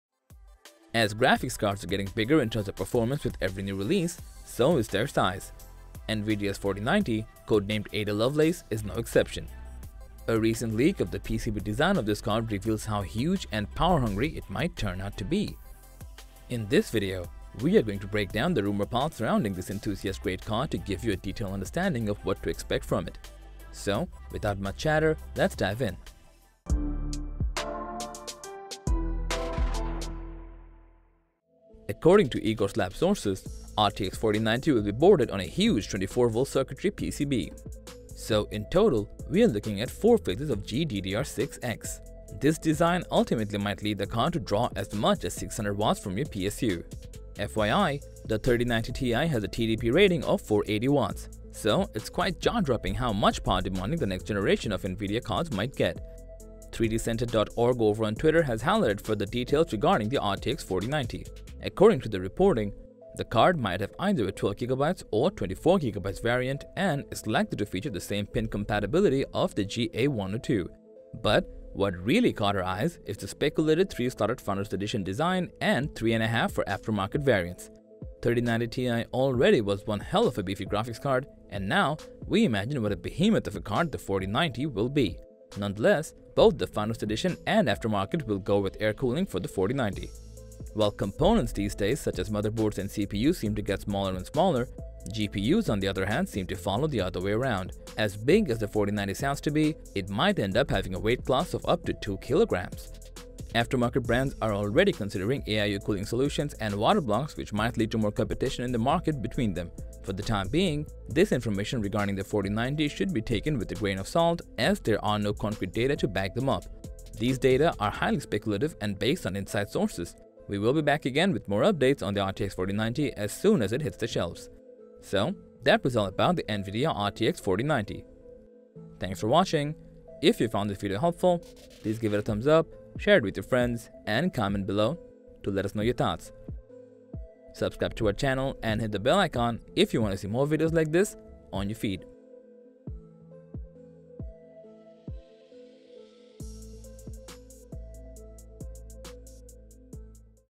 Nvidia RTX 4090 Specs Price & Release Date
Nvidia rtx 4090 specs have just been leaked and we are looking at a massive GPU that hungers for power. It is presumed to be a couple of times faster than the 3090TI while still using the same PCB design. In this video, we break down the rumor pile surrounding the RTX 4090 to give you a detailed idea of what to expect from it.